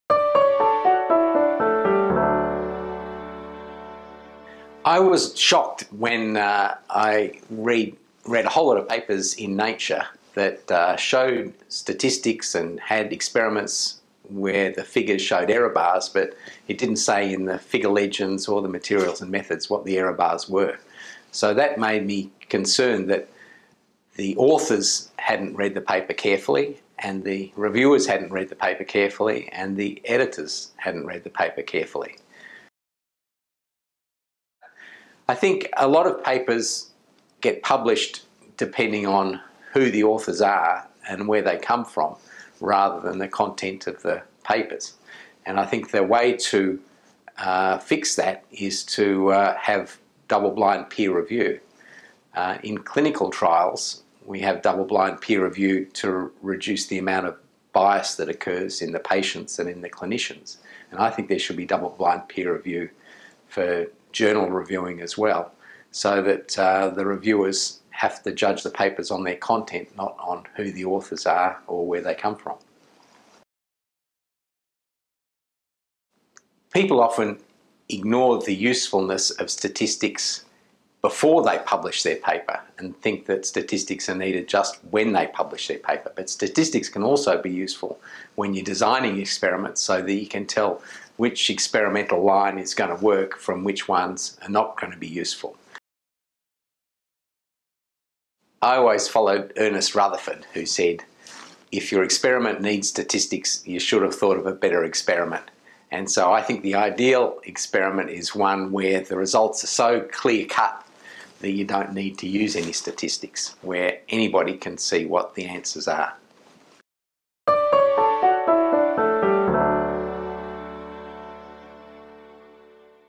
David Vaux talks about publishing in big journals and statistics
When Professor David Vaux from the Walter & Eliza Hall Institute for Medical Research tried to interpret figures from important publications in Nature, he came to a shocking discovery: it was often not stated what the error bars represent. How can reviewers and editors have accepted articles in this world-leading journal without even knowing what the figures mean? Watch this video for his controversial thoughts.